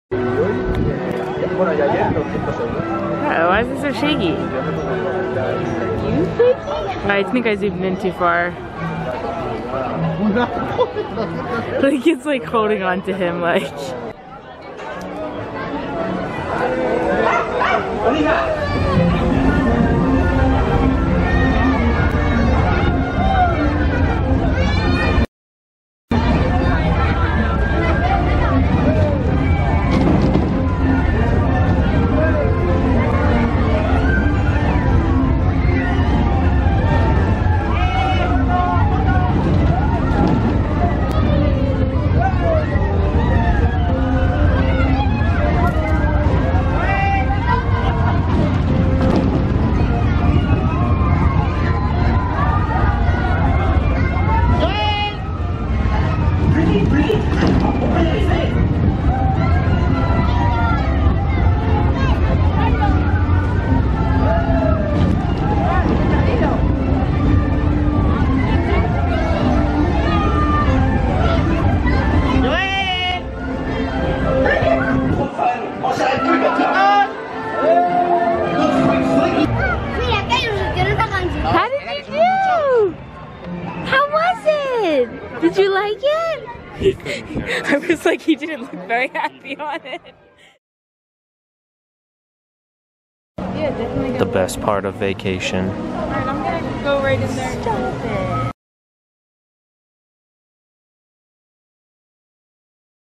Toddler rides Slinky Dog Zigzag Spin (DLP)
Lincoln rides Slinky Dog Zigzag Spin with his Daddy!